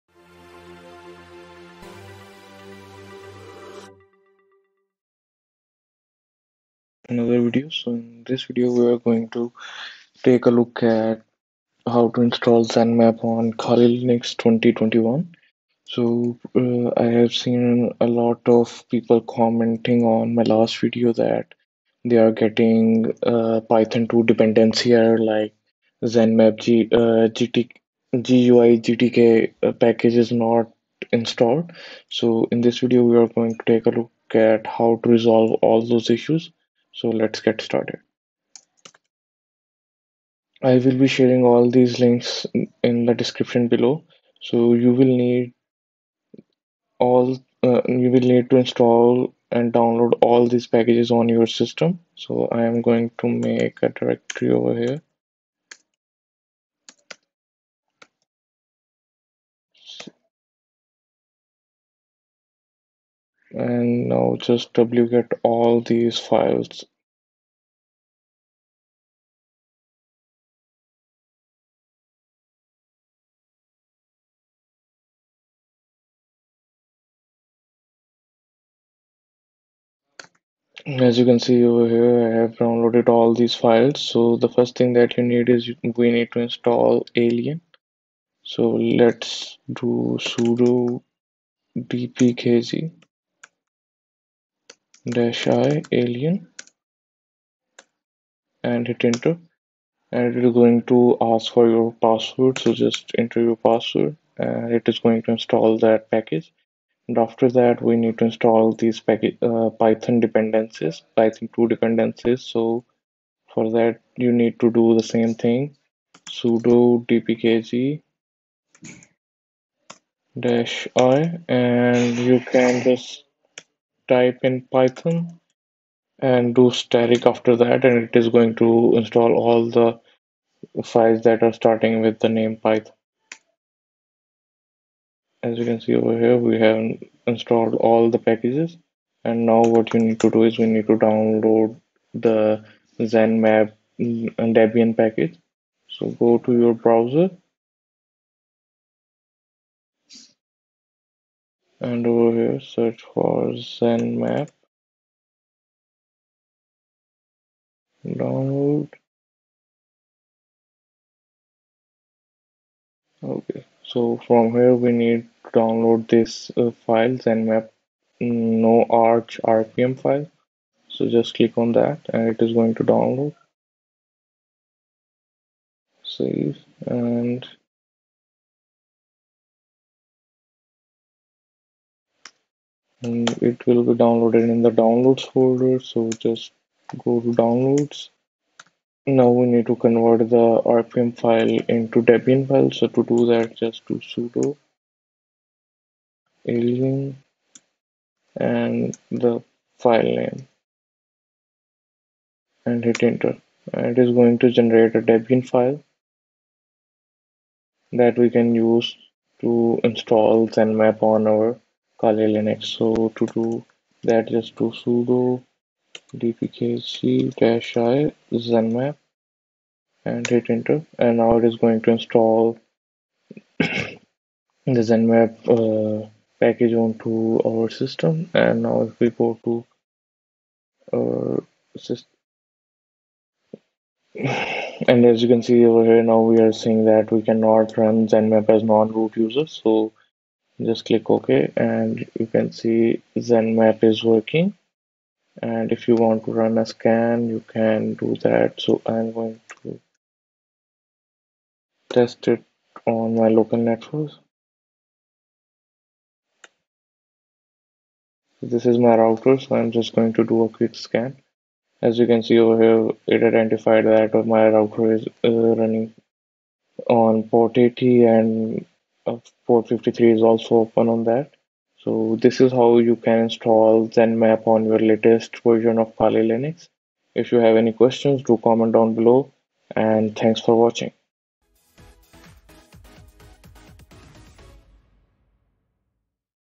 How to easily install Zenmap on Kali Linux 2021 | Could not import the zenmapGUI.App module Fixed
How to easily install Zenmap on Kali Linux 2021 | Could not import the zenmapGUI.App module: 'No module named gtk' fixed.

In this video we will be taking a look at how to install Zenmap on Kali Linux 2021 as Python 2 support has been discontinued we are not able to install zenmap on the latest version of Kali Linux and if we try installing we get the following error: Could not import the zenmapGUI.App module: 'No module named gtk' to fix that we need to install some dependencies that are required by the Zenmap package.

Need a shell to hack from? Set up your own droplet today!